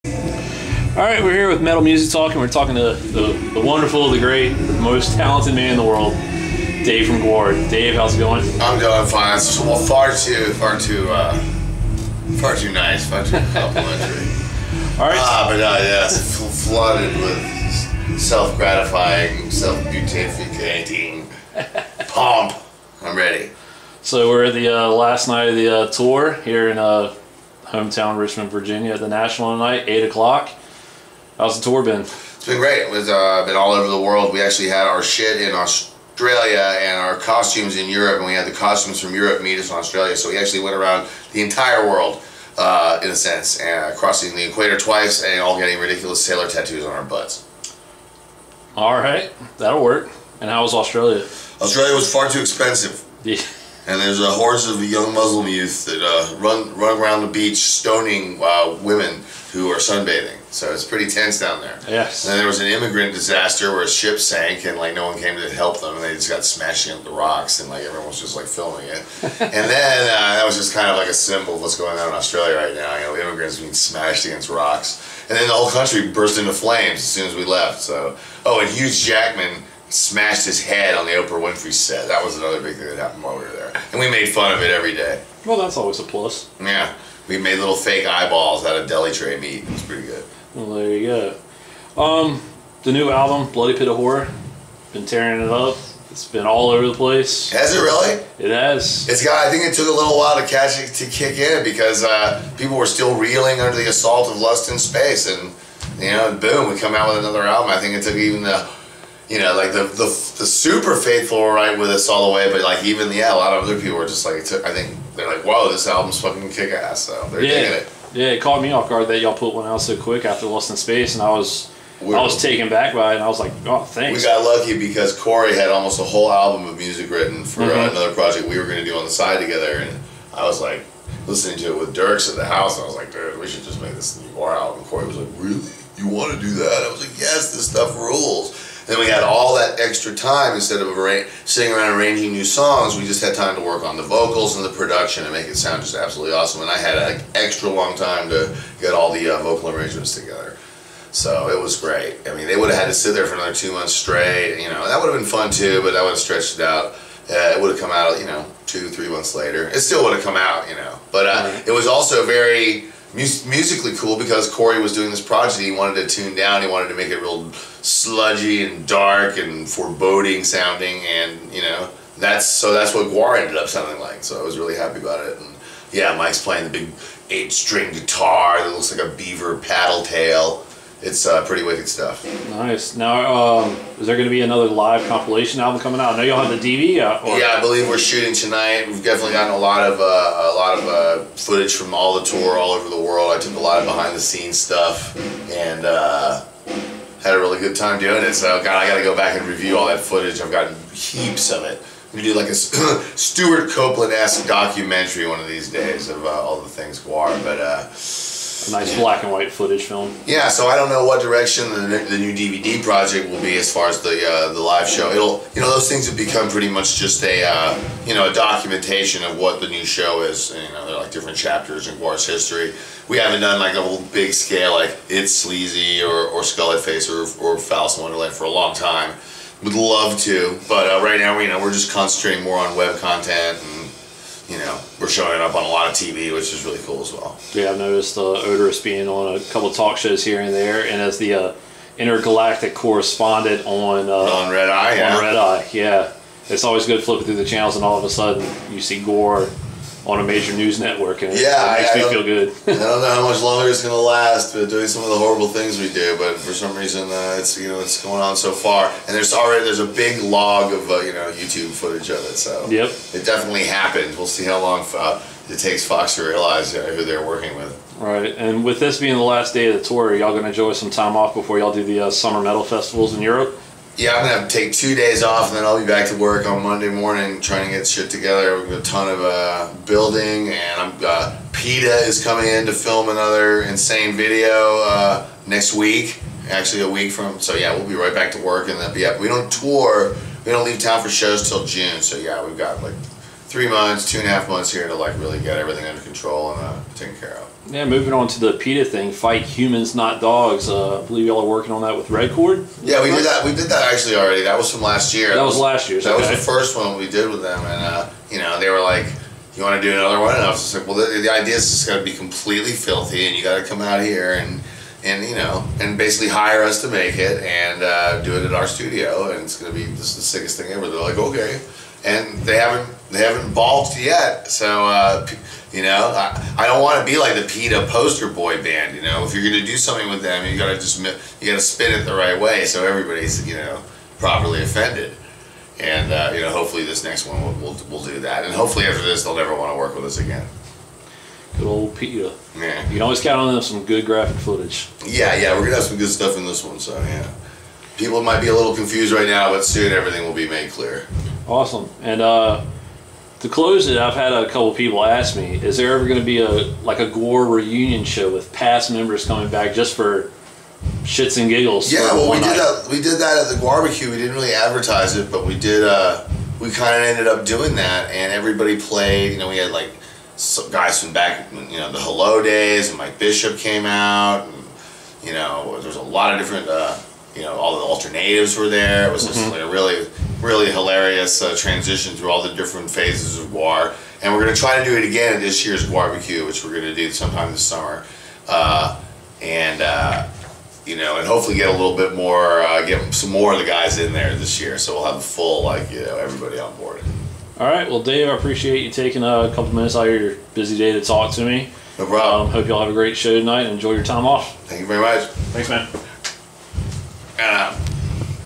Interview with Dave Brockie aka Oderus of GWAR
Great interview with Dave a few hours before the GWAR show at the National in Richmond VA back in March.

RIP DAVE! GWAR WILL NEVER BE THE SAME!!!